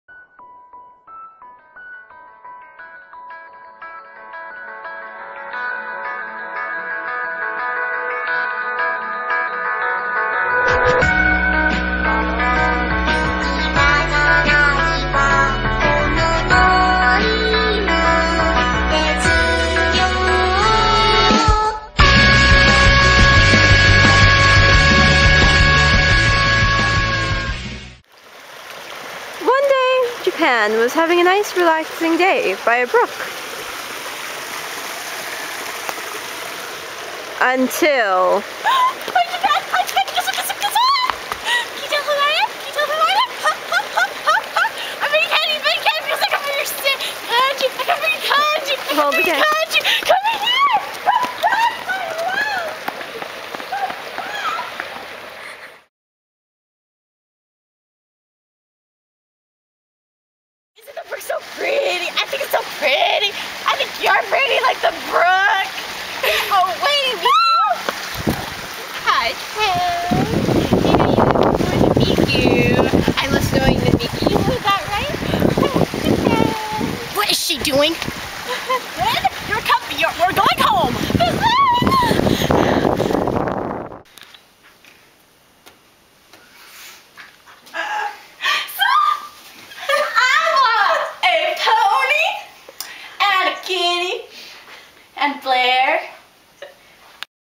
Annoying Japan
This was from the day we went to the park! Sorry about the wind, but it seems every time we go to that park, it's always windy :T